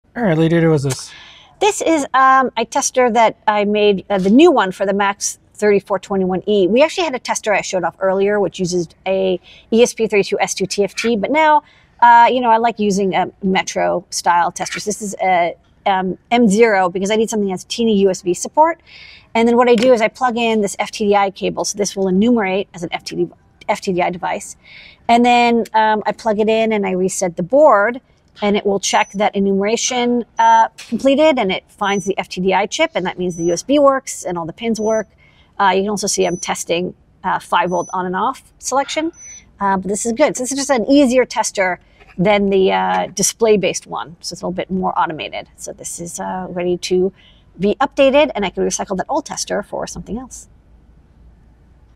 MAX3421e USB Host FeatherWing test 🖥️ 🔌 ✅
We previously made a jerry-rigged USB Host Wing tester based on an ESP32-S2 TFT board and a pogo-pin filled Feather Doubler - but for final testing of hundreds of boards off a panel, a nice custom-made PCB is better. This one uses a Metro M0 since our SAMD21 board support package has TinyUSB support - so we can verify if we enumerate the connected USB peripheral. If a vid/pid is found, we're good to go, and beep and blink to indicate the test has passed.